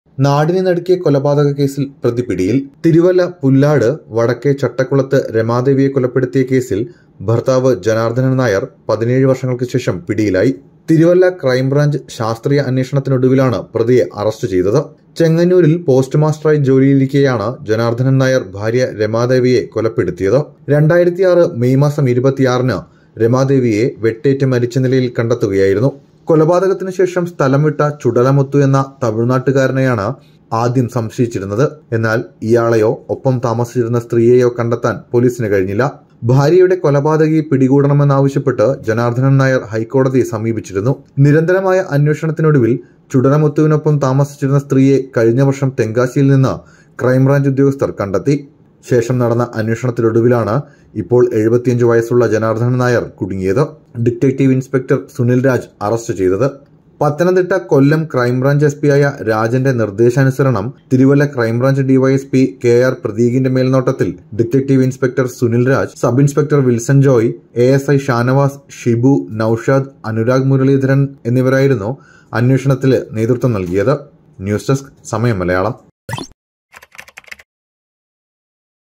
രമാദേവി കേസിൽ പ്രതി പിടിയിൽ | Ramadevi Case | Thiruvalla police
ഭാര്യയുടെ കൊലപാതകത്തിൽ പ്രതികളായവരെ പിടികൂടണമെന്നാവശ്യപ്പെട്ട് ഇയാൾ ഹൈക്കോടതിയെ സമീപിച്ചിരുന്നു